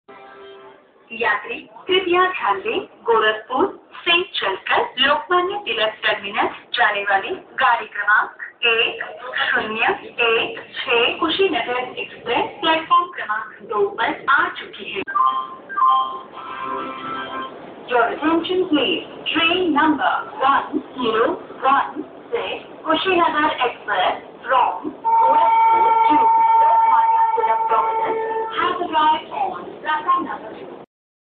Kushinagar Express Train RARE Announcement at Bina Railway Station !!!
Train Announcement for 11016 Gorakhpur - Lokmanya Tilak Nagar Terminus Kushinagar Express Train at Bina Junction